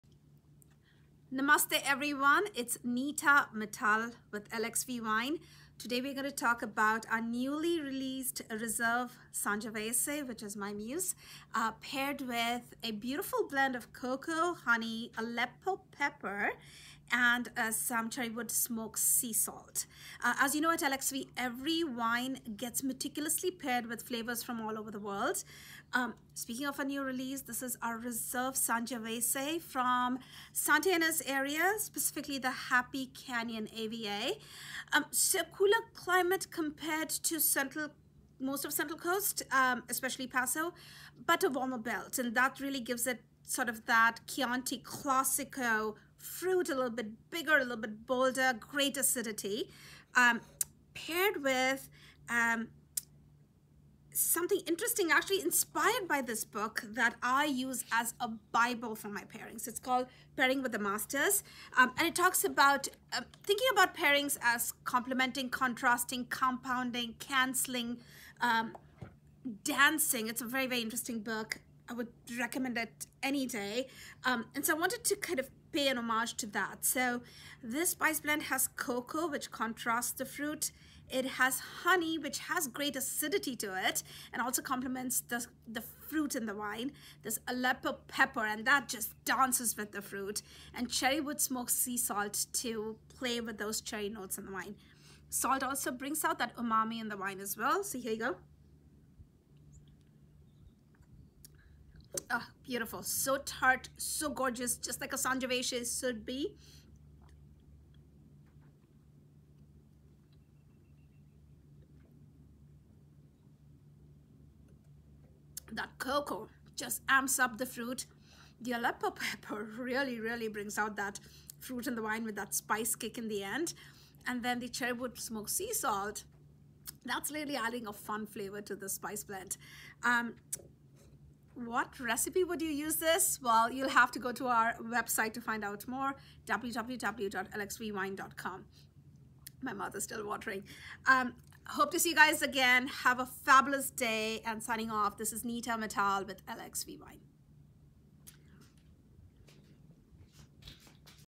LXV 2019 Reserve Sangiovese Spice Pairing: Crescendo
Neeta Mittal walks us through yet another unique LXV wine and spice pairing: 2019 Reserve Sangiovese & Crescendo spice blend.